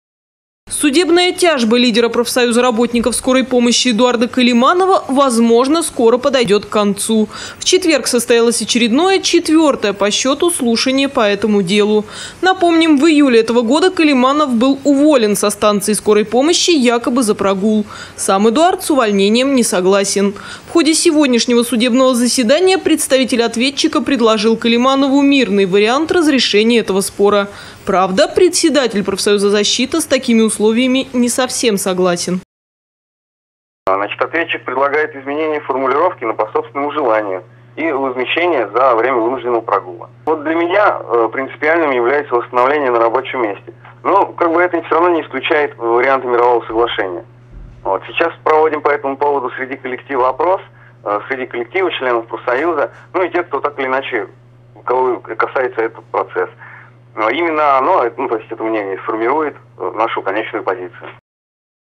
Каляманов пока не согласен на мировую...
Судебная тяжба лидера профсоюза работников скорой помощи Эдуарда Каляманова, возможно, скоро подойдет к концу. В четверг состоялось очередное, четвертое по счету слушание по этому делу.